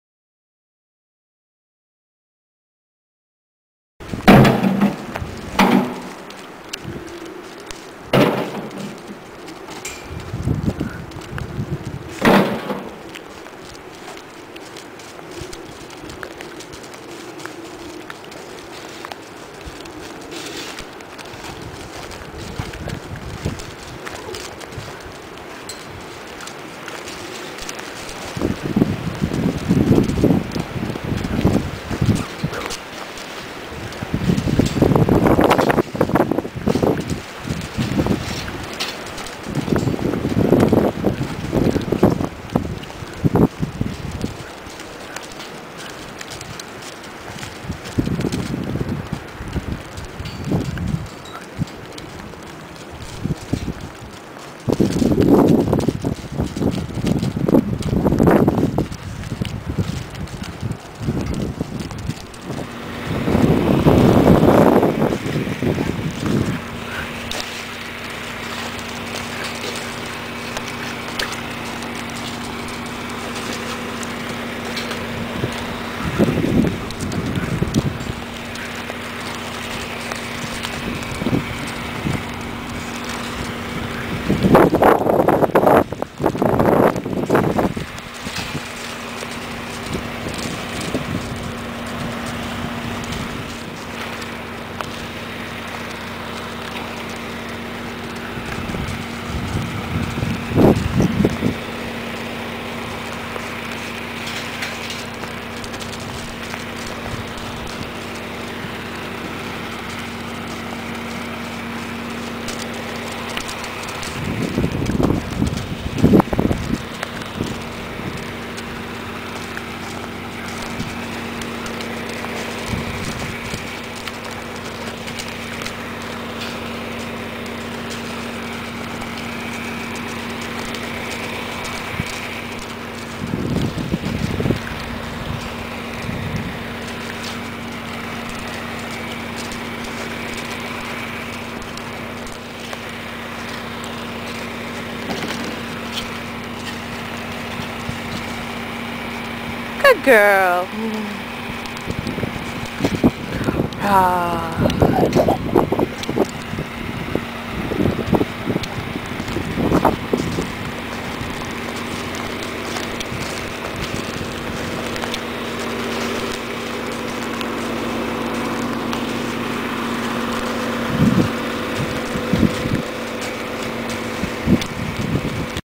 Cookie Free Jumping
Cookie, my mare free jumping in the round pen. Yes, and don't you just love my homemade jump?